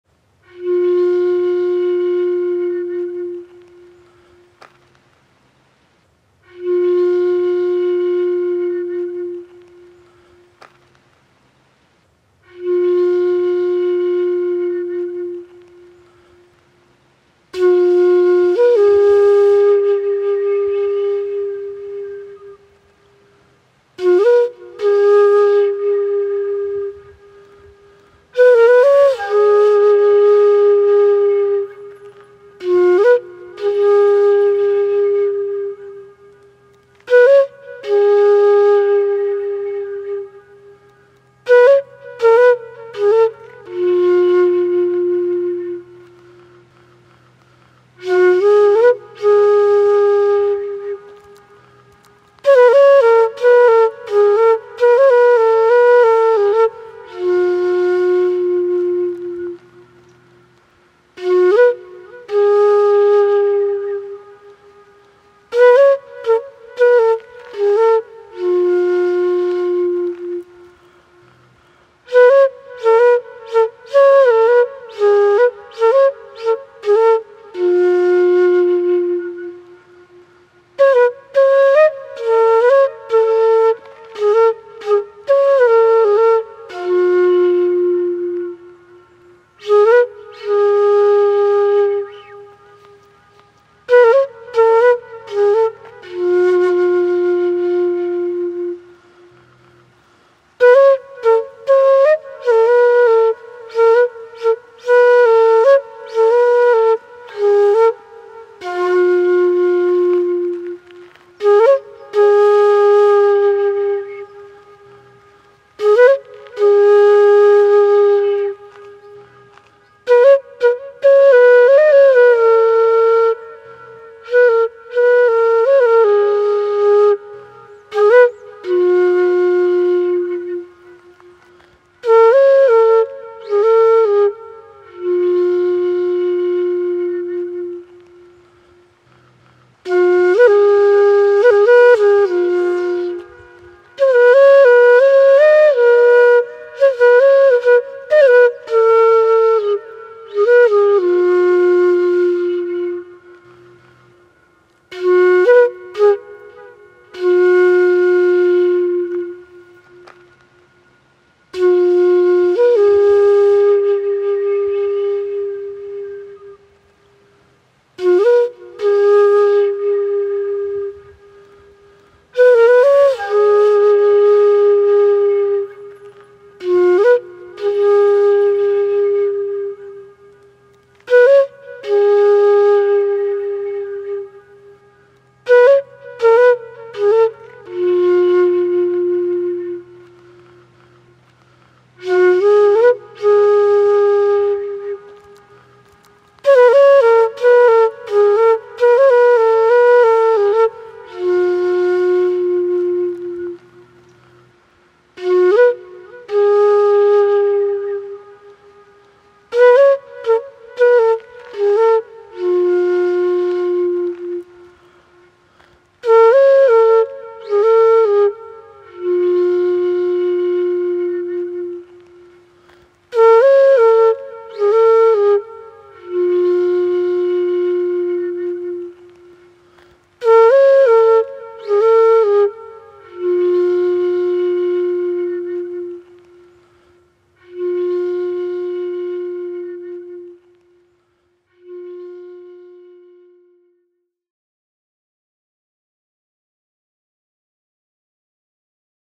Věžka: Improvizace na bansuri flétnu
Na zřícenině hradu Věžka jsem si užíval samotu s flétnou a nahrál několik krátkých improvizací. Jednu z nich  si můžete poslechnout v tomto videu a zároveň si užít záběry z Věžky.